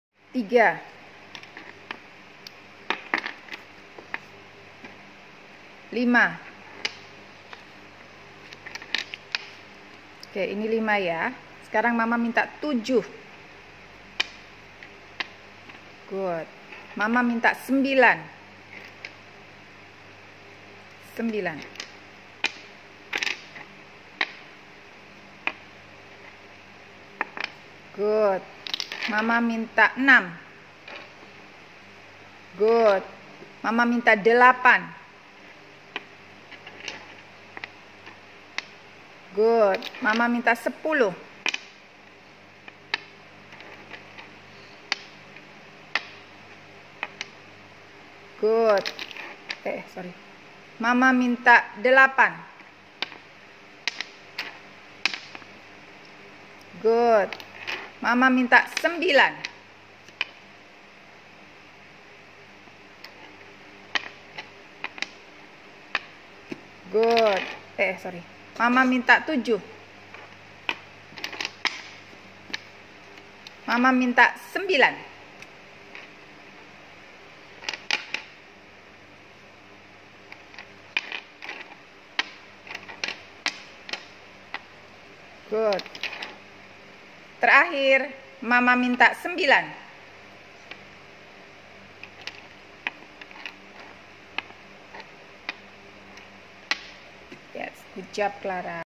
Mental Math - Number Sense 2
kali ini mama minta clara yg menaruh barang sejumlah bilangan yg disebutkan. berbeda dg latihan 1 mama yg memainkan benda clara yg menyebutkan tanpa menghitung satu per satu

Targetnya Clara sdh dapat menaruh benda sesuai bilangan yg disebutkan tanpa menghitung tp mengenal pola

latihan dg 10 frame